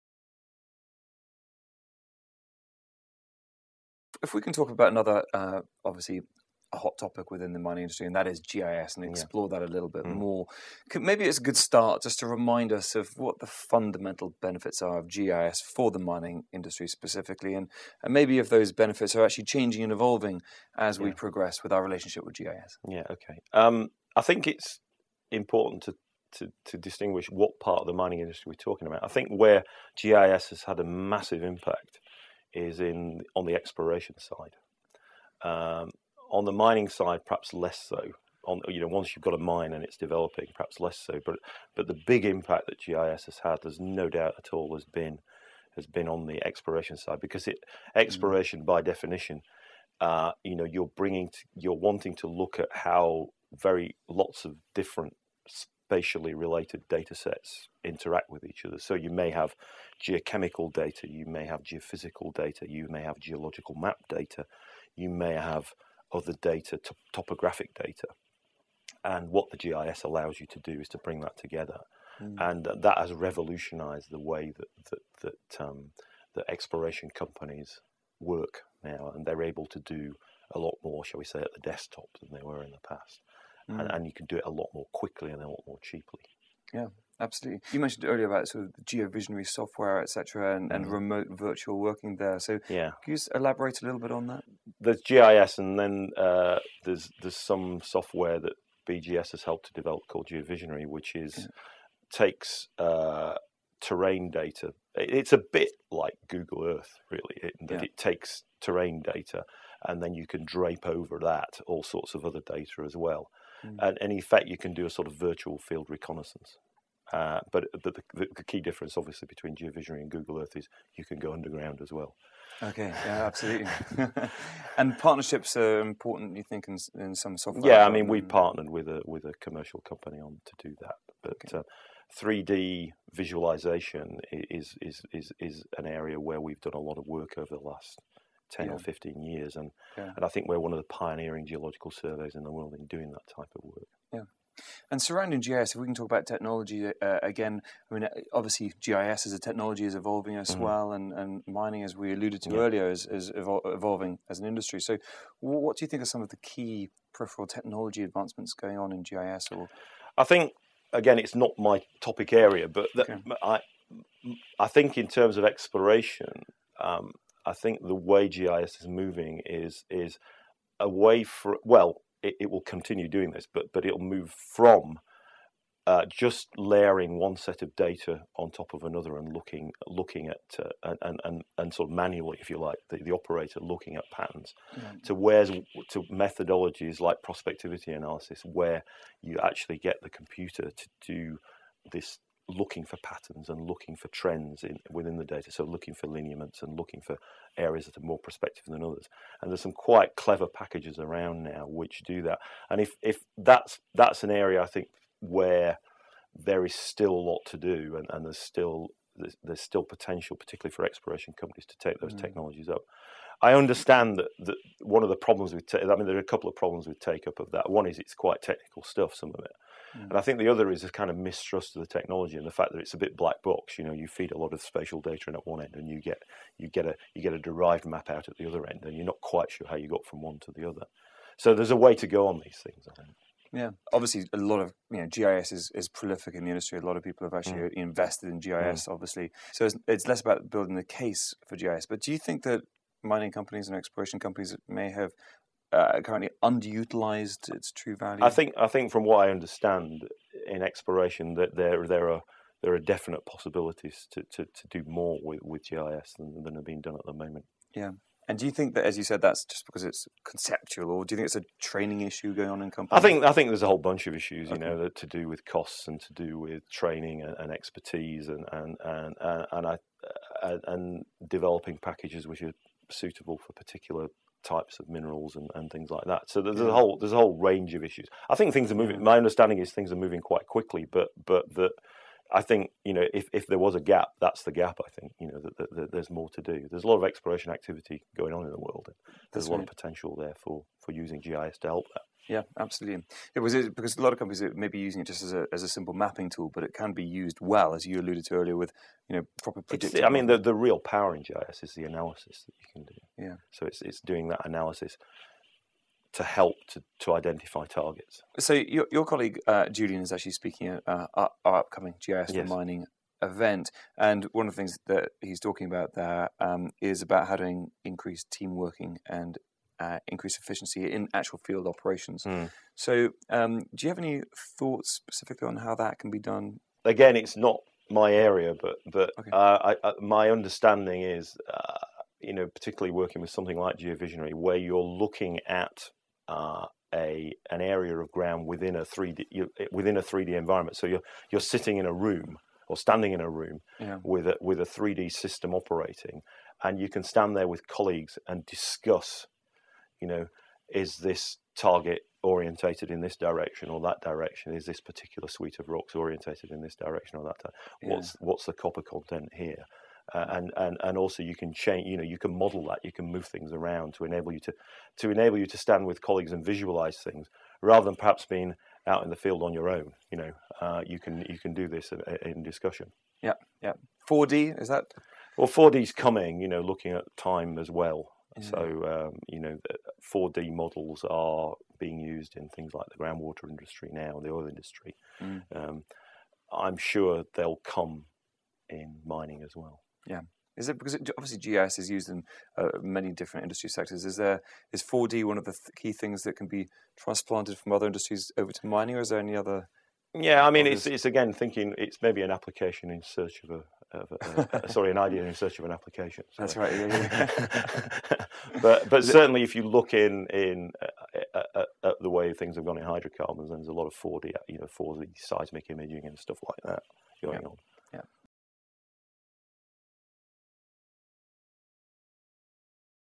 An overview of GIS for mining and exploration - with Andrew Bloodworth
Andrew Bloodworth, Head of Science for Minerals & Waste for the British Geological Survey, is a specialist in geology, mineralogy, and mining investment. He talks with Mining IQ about the industry as he sees it, from understanding how public perception of mining can affect investment and where he sees the future geological landscape shift across the whole of this century. If you have any interest in the field, this is a must-watch analysis. In this interview, Andrew gives an overview of GIS for mining and exploration, looking at its uses, changing benefits and technological innovations.

For more information about Mining IQ and for the rest of this interiview, please